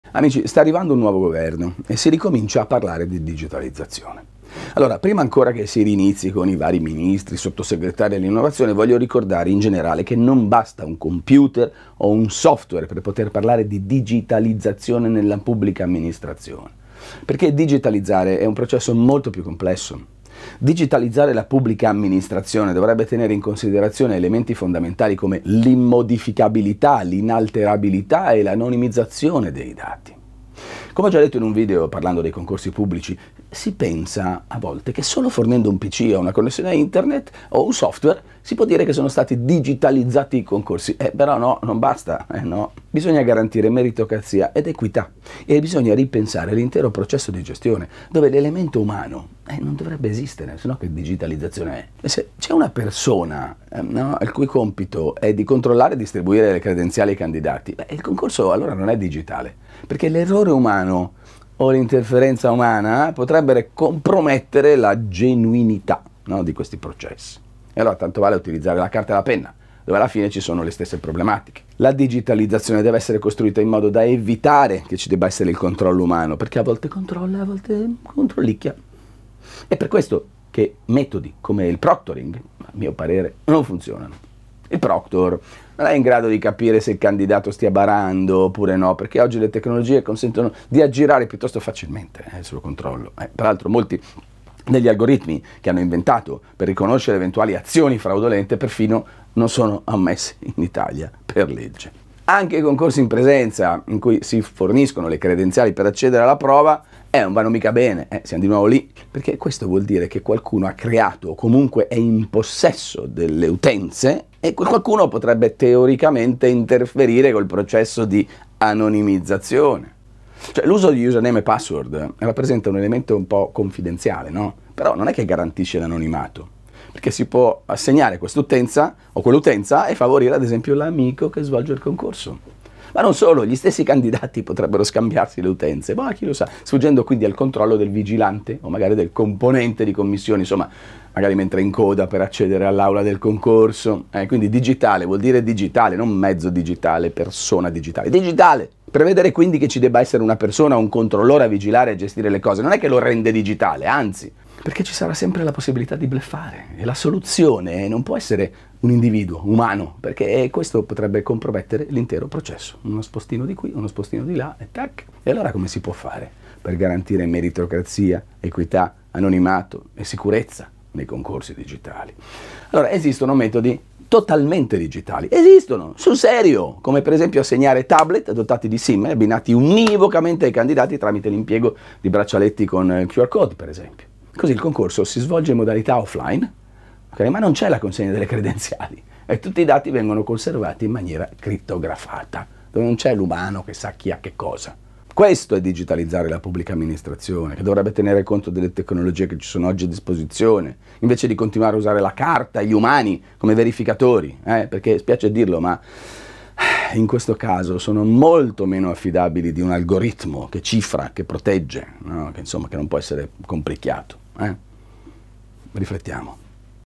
Non basta un computer per digitalizzare la pubblica amministrazione
Non basta un computer o un software per poter parlare di digitalizzazione nella pubblica amministrazione.
Digitalizzare è un processo molto più complesso.

Per esempio nei concorsi pubblici si pensa che solo fornendo un PC con una connessione internet, o un software, si possa dire che sono stati digitalizzati. Ma non è così.